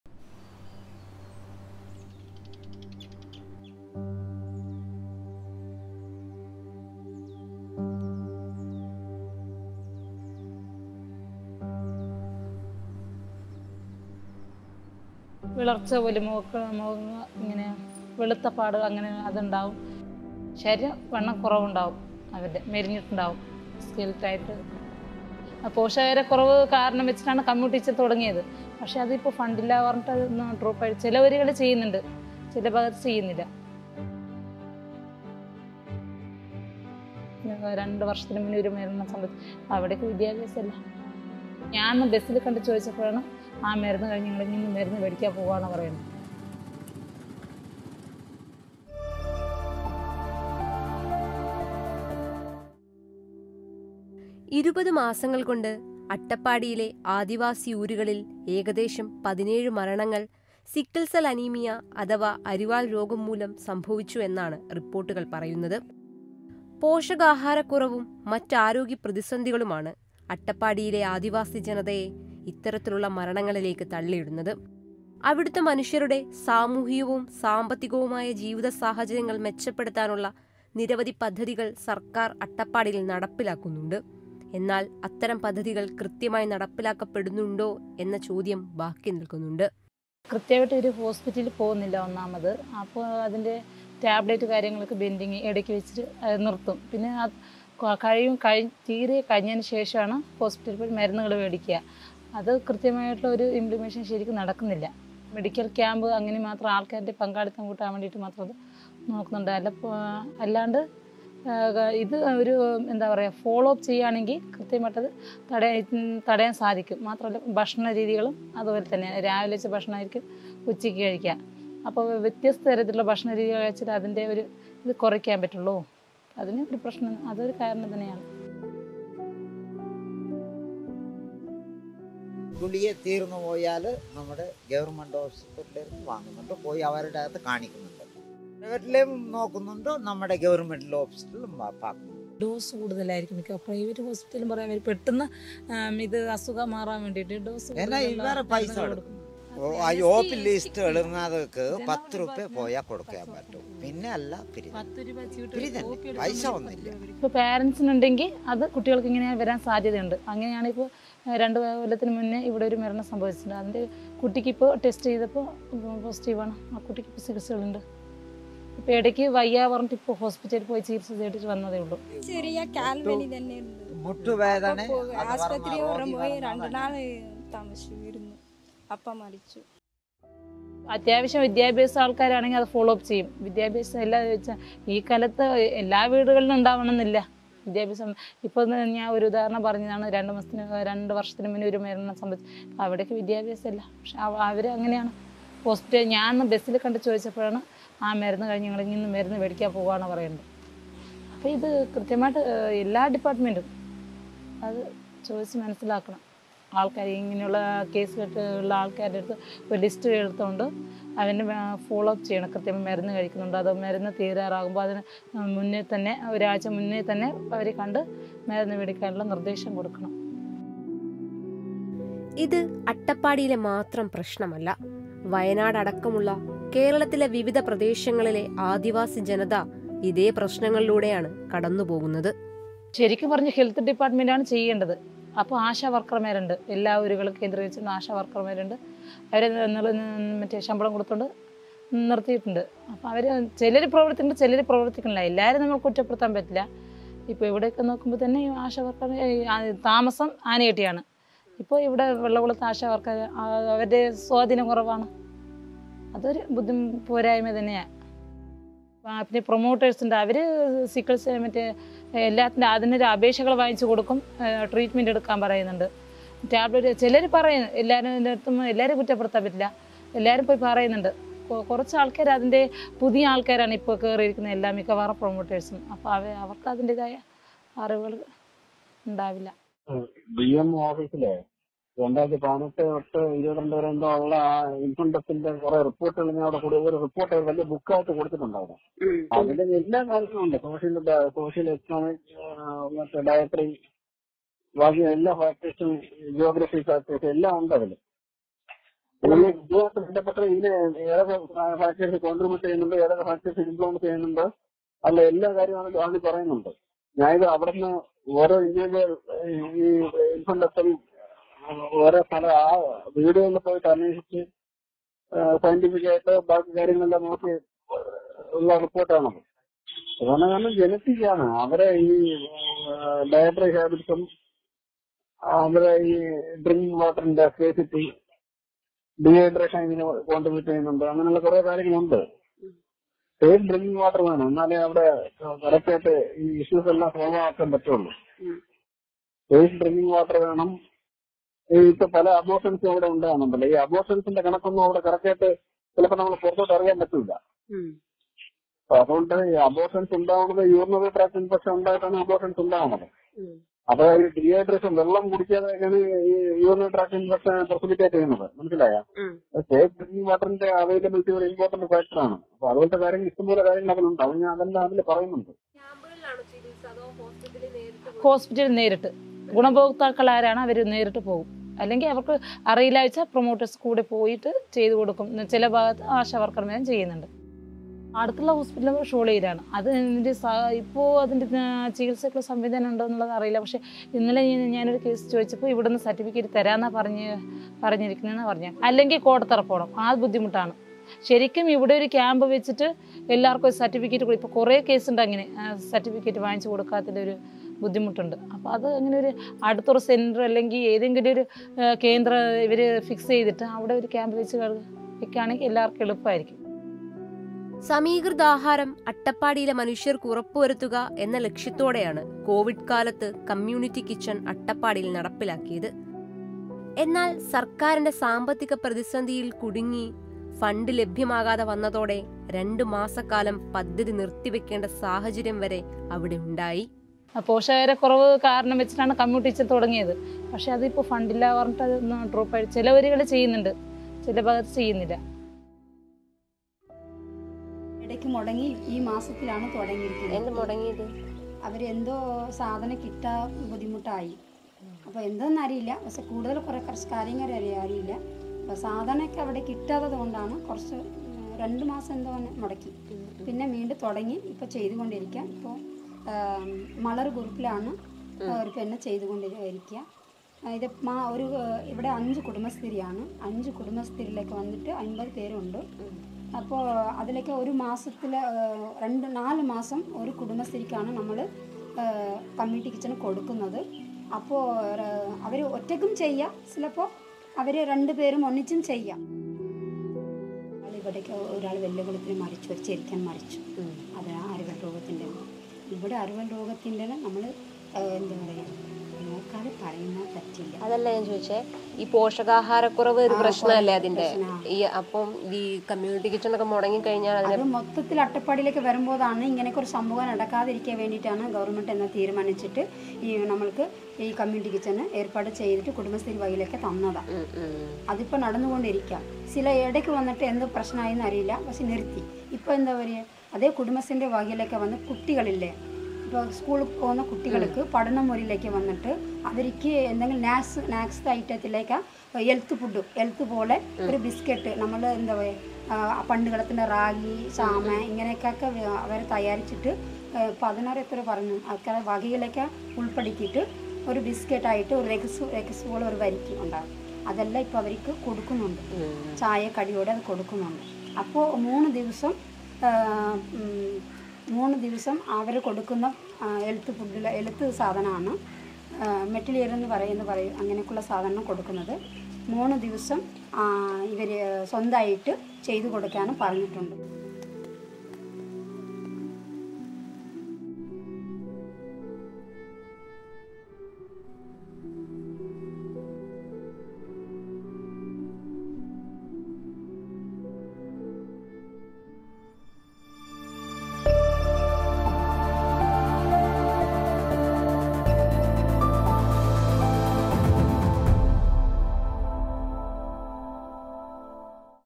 2 വര്‍ഷത്തിനുള്ളില്‍ 17 മരണം; അരിവാള്‍ രോഗത്തോട് പൊരുതുകയാണ് Attappadi | Sickle cell anemia
ഇരുപത് മാസങ്ങള്‍ക്കൊണ്ട് അട്ടപ്പാടി ആദിവാസി ഊരുകളില്‍ 17 മരണങ്ങള്‍ സിക്കിള്‍ സെല്‍ അനിമീയ അഥവാ അരിവാള്‍ രോഗംമൂലം സംഭവിച്ചതെന്നാണ് മാധ്യമങ്ങള്‍ റിപ്പോര്‍ട്ട് ചെയ്യുന്നത്. പോഷകാഹാരക്കുറവും മറ്റ് ആരോഗ്യ പ്രതിസന്ധികളുമാണ് അട്ടപ്പാടിയിലെ ആദിവാസി ജനതയെ ഇത്തരത്തിലുള്ള മരണങ്ങളിലേക്ക് തള്ളിയിടുന്നത്. അവിടുത്തെ മനുഷ്യരുടെ സാമൂഹ്യവും സാമ്പത്തികവുമായ ജീവിതസാഹചര്യങ്ങള്‍ മെച്ചപ്പെടുത്താനുള്ള നിരവധി പദ്ധതികള്‍ സര്‍ക്കാര്‍ അട്ടപ്പാടിയില്‍ നടപ്പിലാക്കുന്നുണ്ട്. എന്നാല്‍ അത്തരം പദ്ധതികള്‍ കൃത്യമായി നടപ്പിലാക്കപ്പെടുന്നുണ്ടോ എന്ന ചോദ്യം ബാക്കി നില്‍ക്കുന്നുണ്ട്.

According to media reports, 17 deaths have occurred in the Attappadi tribal villages over the past twenty months due to sickle cell anemia. Malnutrition and other health crises are pushing the tribal population of Attappadi towards such fatalities. The government is implementing several schemes to improve the social and economic conditions of the people there. However, questions remain as to whether these schemes are being properly executed.